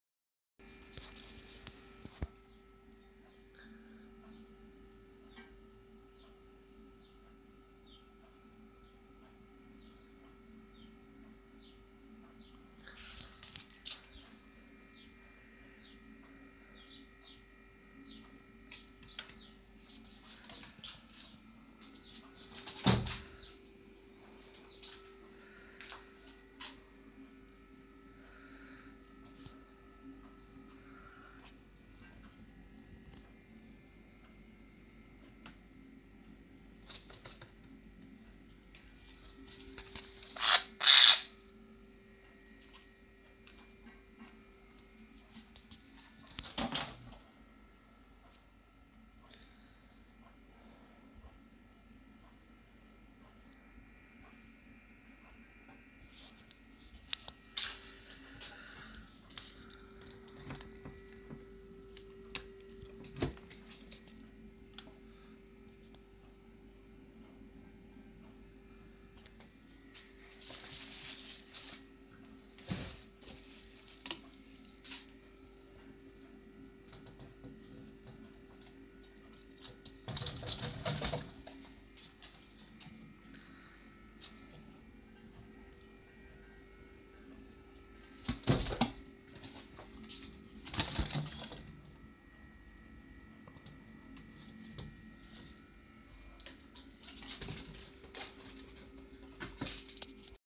Młoda sójka w domu, siedziała na srodku ulicy w upał
ten ptak to prawdopodobnie sójka.Kiedy jechałam samochodem siedział z rozłozonymi skrzydłami i otwartym dziobem na rozgrzanym przez wiosenne słońce asfalcie. zastanawiałam się czy go zostawić, ale moim zdaniem sam nie dałby rady bo był zbyt zmęczony,lub zginąłby pod kołami samochodu.Gdy wzięłam go do domu odpoczął i zaczął być żywotny.nakarmiłam go truskawkami z kompotu bo tylko to chciał jeść a właściwie i pić, na drugi dzien rano wypusciłam go w tym samym miejscu gdzie go znalazlam, podrzuciłam do góry i poleciał na pobliskie drzewo w lesie, więc dał sobie radę :)